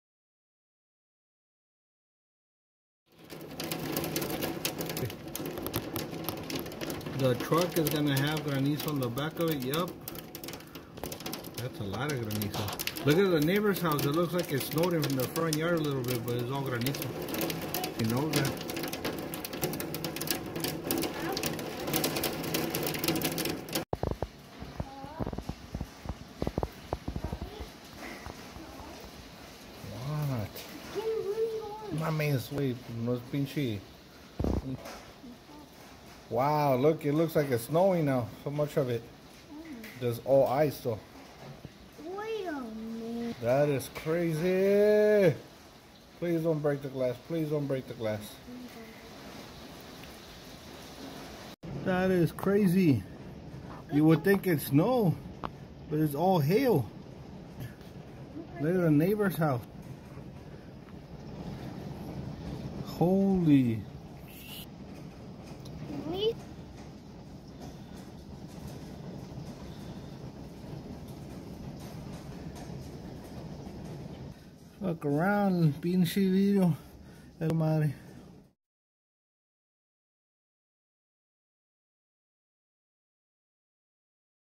10-07-2022 Tucson, AZ - Severe T-Storm~Accumulating and Damaging Hail Hit Tucson Suburb, Stationary
Severe-warned thunderstorm produced very large and damaging hail in the Sahuarita suburb, just south of Tucson this afternoon.  This storm was extremely slowing moving (basically stationary) and was only moving at 2 km/h.  This video was taken from a homeowner showing how quickly the hail accumulated.  The temperatures dropping significantly and it felt like Autumn.

It looked like a Winter Wonderland, within, just a few seconds.  The hail reached, at least 1.50”+ in diameter.  After second :23, the screen goes full and at the :50 second mark, you can see how everything is covered in hail.  It looks like a scene out of a Christmas story.  Thankfully, school had let out early afternoon, before this storm hit.